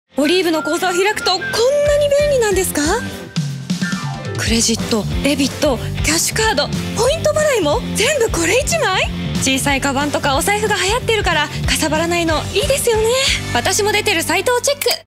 【Olive】Oliveな人 宇垣美里さん篇_デザイン改訂版_15sec_縦
【Olive】推す人、どんどん増えてます

Oliveアカウントを開設すれば、
口座、クレジット、デビット、ポイント払い、さらには保険・証券まで、
1つのアプリでまとめて管理できる。